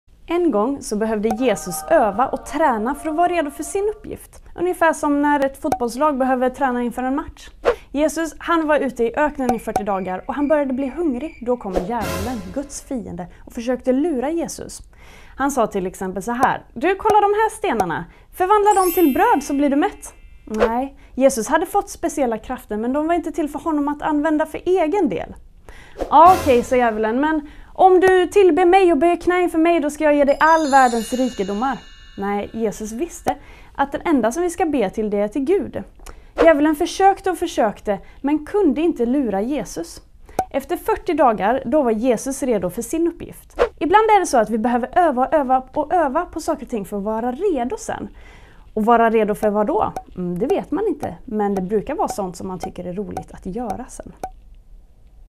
Youngsters minut - Jesus frestas
En minut med Youngsters där Hanna berättar om när Jesus frestades av djävulen i öknen